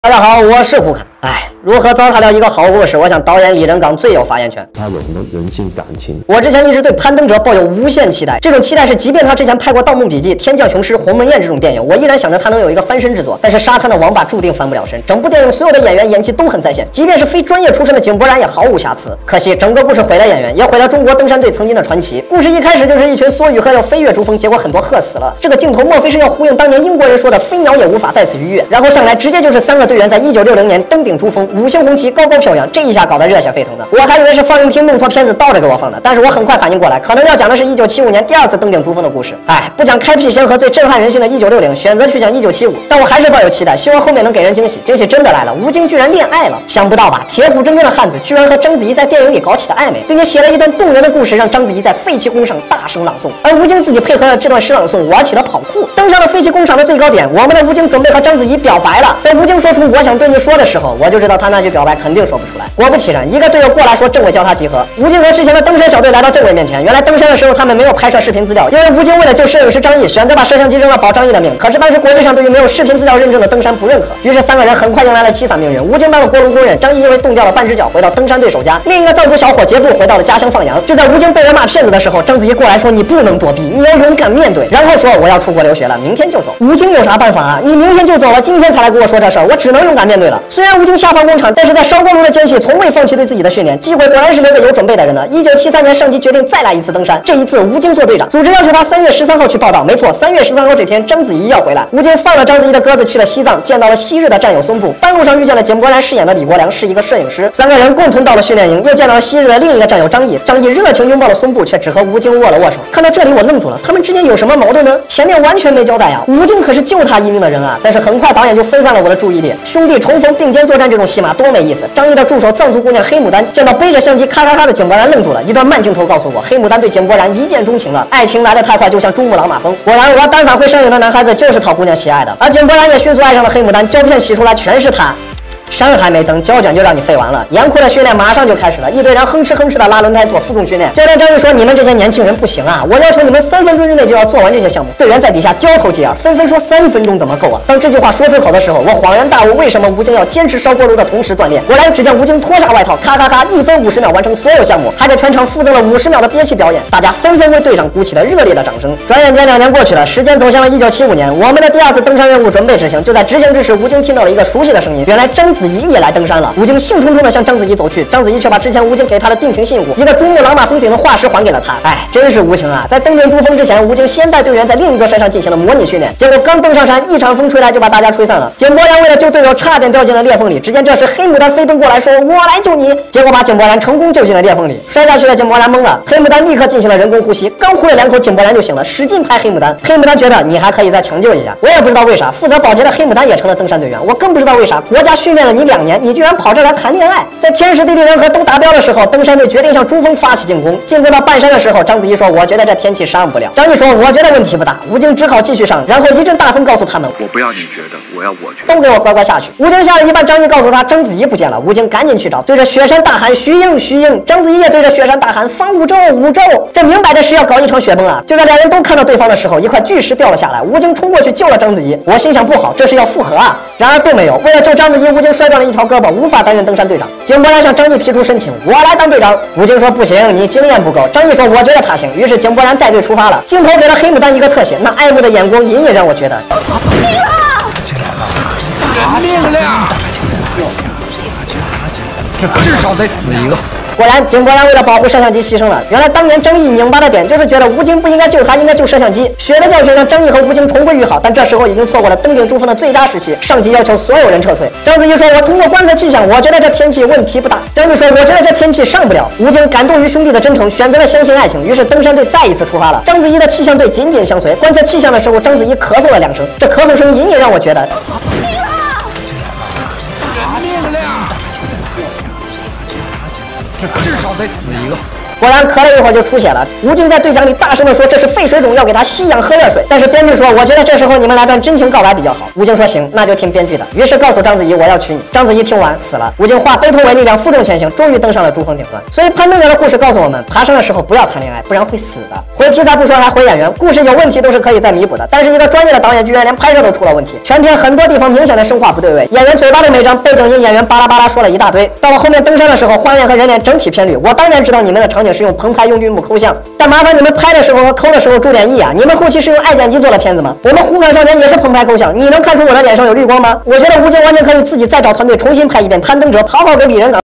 【内有剧透】李仁港导演用《攀登者》告诉我们：爬山的时候谈恋爱会出人命！
我之前一直对《攀登者》抱有无限期待，这种期待是即便他之前拍过《盗墓笔记》、《天将雄师》、《鸿门宴》这种电影，我依然想着他能有一个翻身之作，但是沙滩的王八注定翻不了身。整部电影所有的演员演技都很在线，即便是非专业出身的井柏然也毫无瑕疵，可惜，整个故事毁了演员，也毁了中国登山队曾经的传奇。